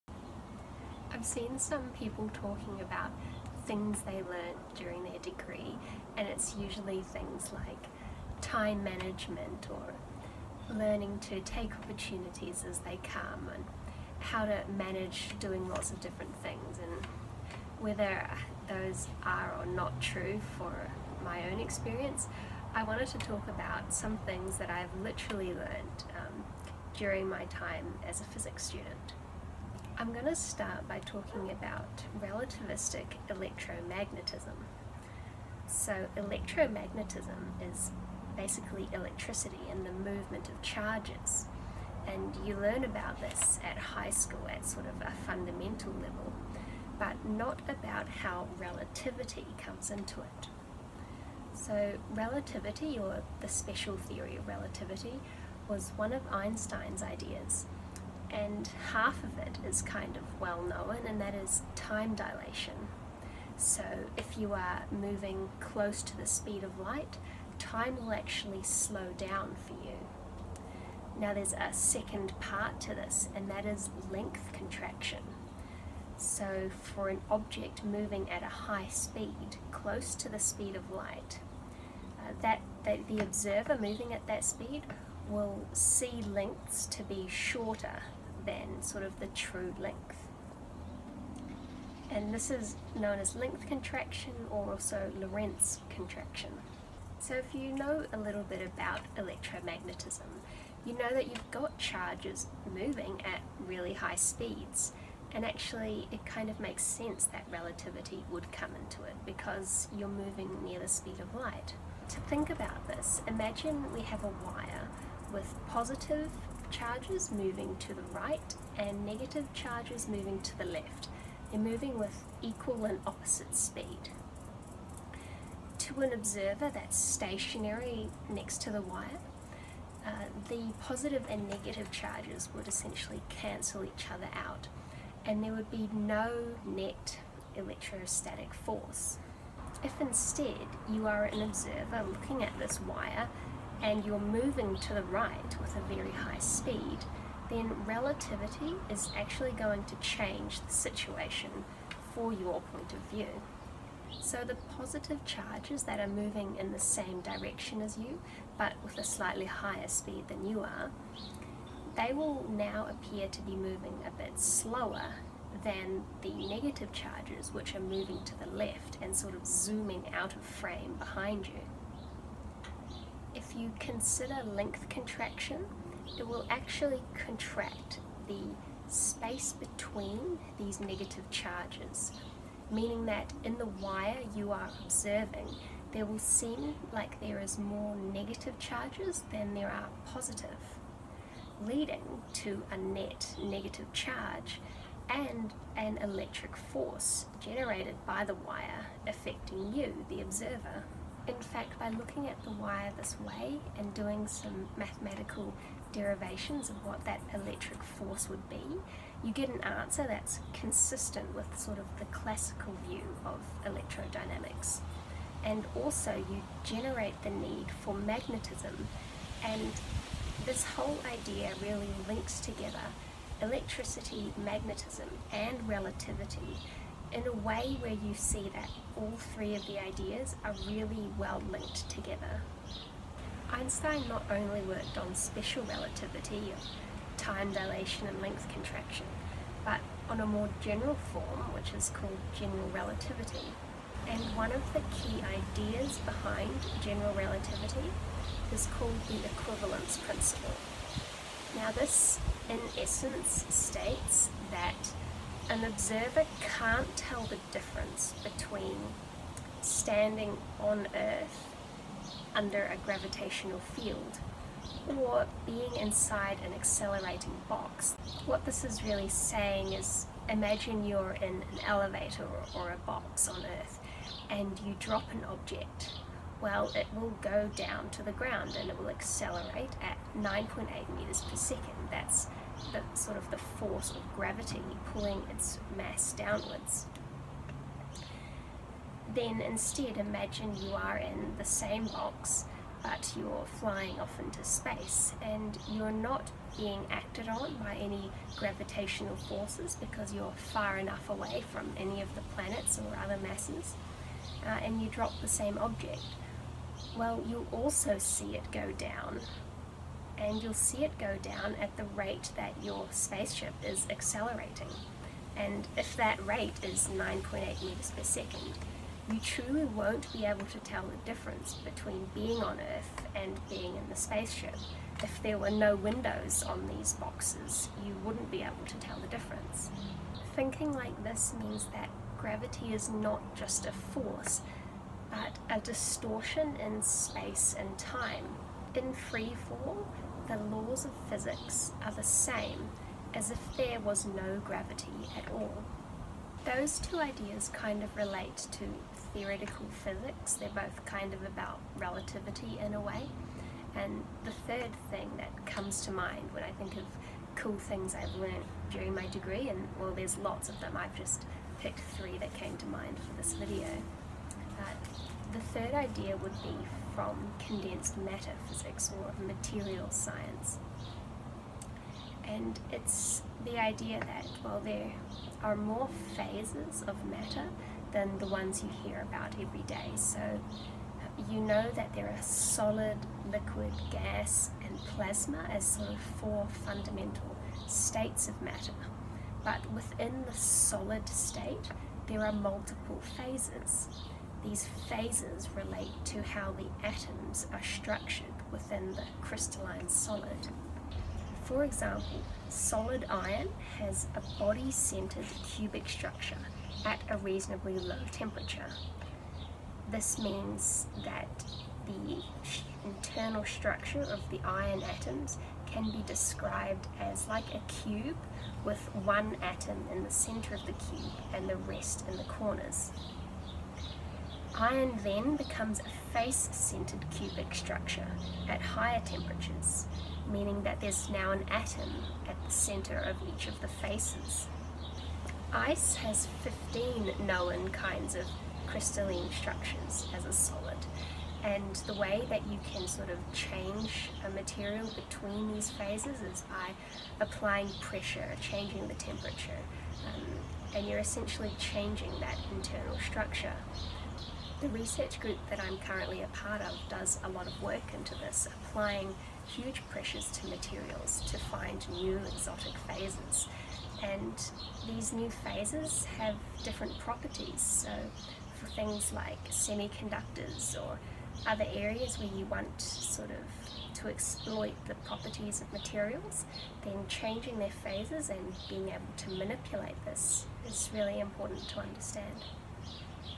3 things I (actually) learnt during a physics degree
I attempt to explain a few of the things that I remember enjoying during my physics studies.
In the relativistic electromagnetism example I miss out a technicality and should have mentioned that I am co-moving or stationary with respect to a charged particle. If you want clarification on this please ask. I could do a math version of this if anyone is interested.